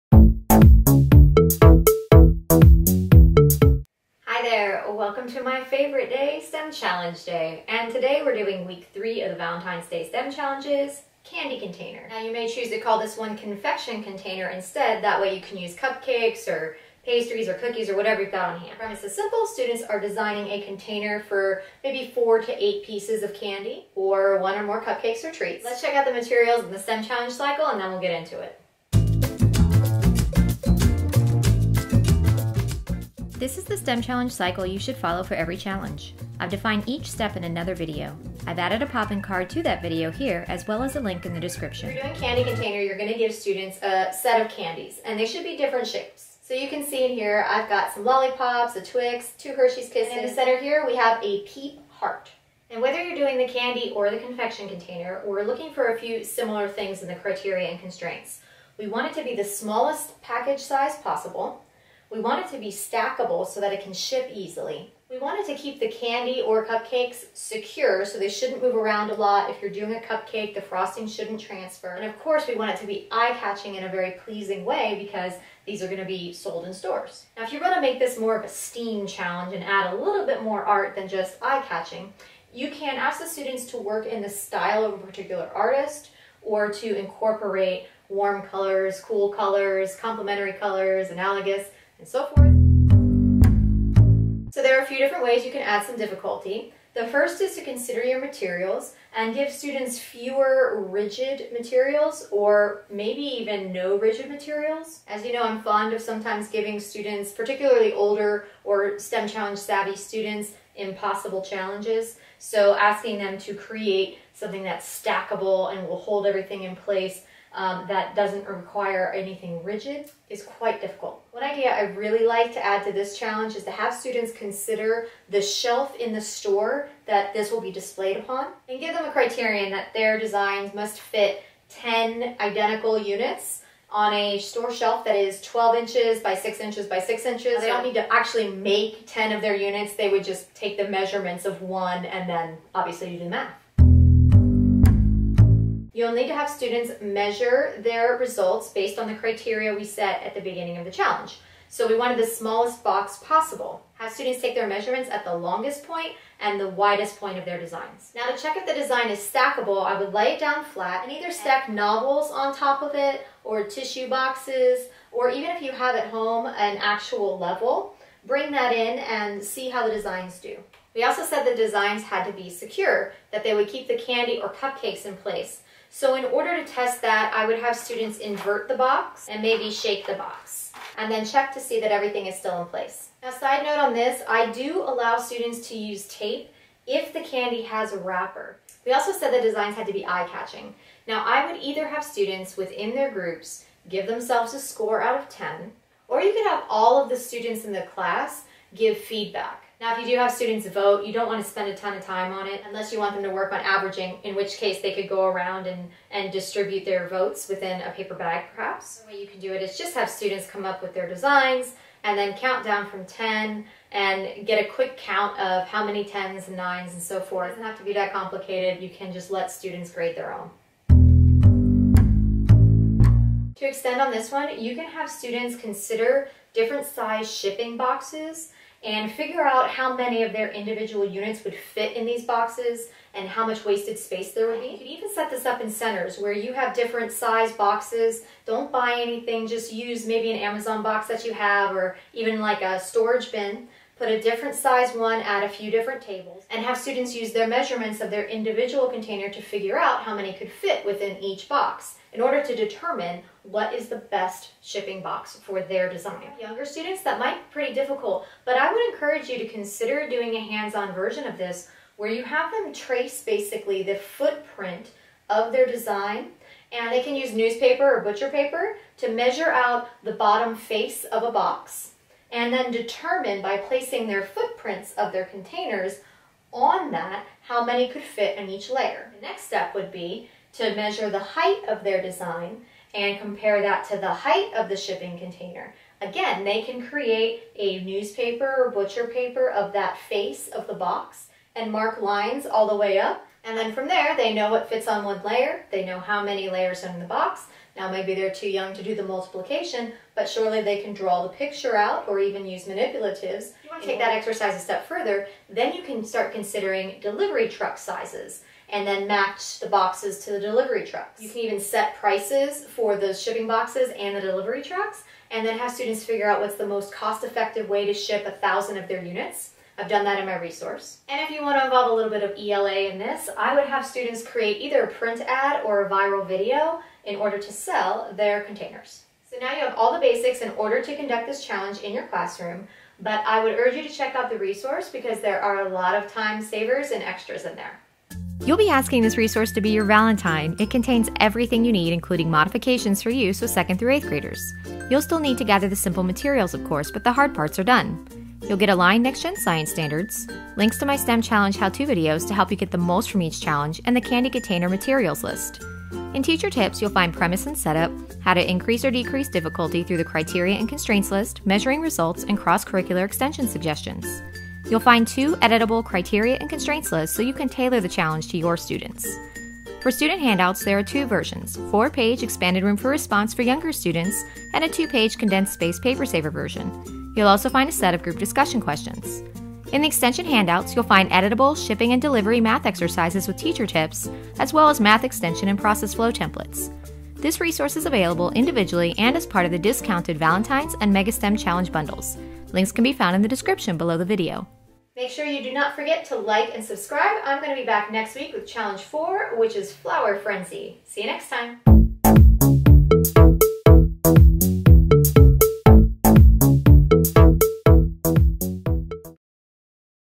Valentine’s Day STEM Challenge: Candy / Confection Container
Valentine’s Day STEM Challenges are a great way to keep students engaged, thinking critically, and working on hands-on problem solving and they’re great alternatives to class parties! In Candy/Confection Container, students create a container that keeps its contents secure and is small, stackable, and aesthetically pleasing!